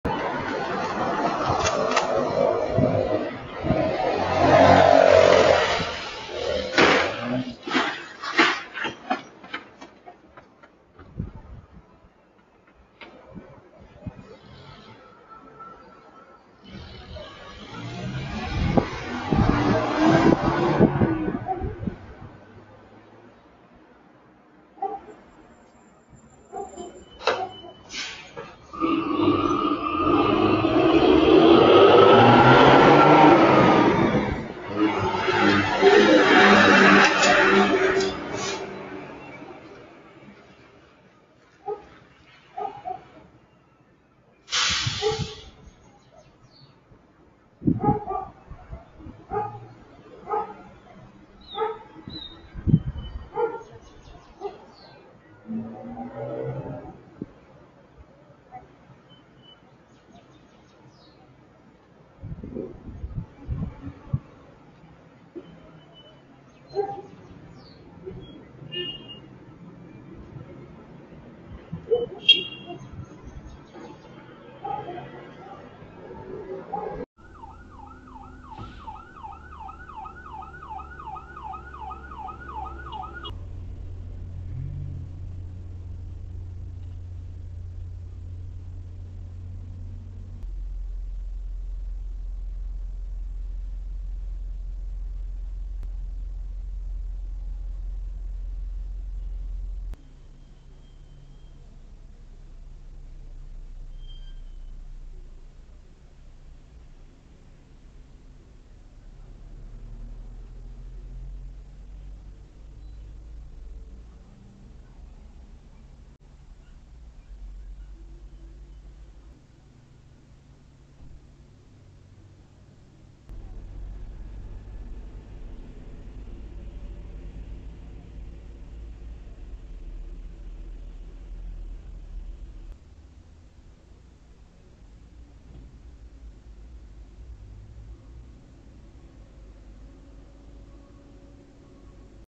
Violento choque entre motocicleta y jeep
Esta mañana, en la avenida Claudio Vallejo (kilómetro 38,2 de la vía Cuenca Girón Pasaje) y calle Eloy Alfaro del cantón Girón, se produjo un violento choque entre una moto y un jeep, dando como resultado que las dos personas ocupantes de la motocicleta, un hombre y una mujer mayores de edad, sufran fracturas en sus extremidades inferiores, según reportaron Bomberos Girón que acudieron a atender la emergencia. El hecho quedó grabado en una cámara de videovigilancia.